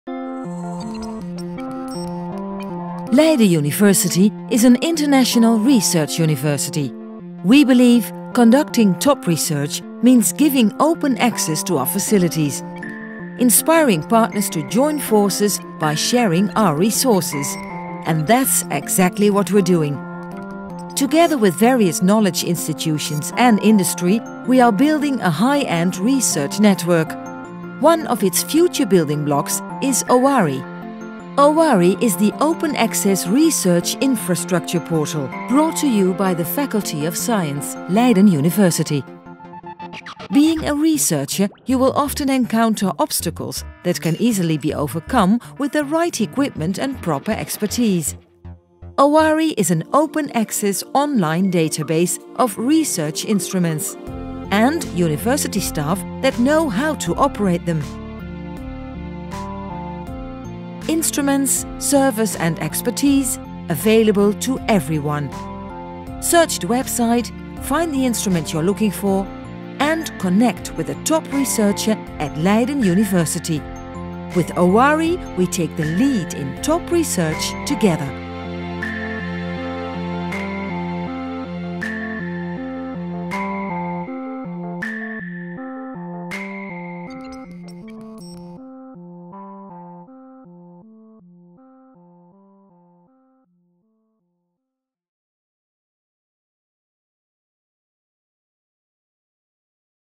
OARI - Open Access Research Infrastructure
The Faculty of Science opens up its research facilities by introducing the 'Open Access Research Infrastructure' (OARI) portal. The portal contains an online database with available research instruments and related expertise. The instruments are accessible to researchers of universities, other knowledge institutions and industry.

Please visit OARI at oari.science.leidenuniv.nl and connect with our researchers. Furthermore, if you’re interested in sharing your own publicly funded research infrastructure, we welcome you to join OARI.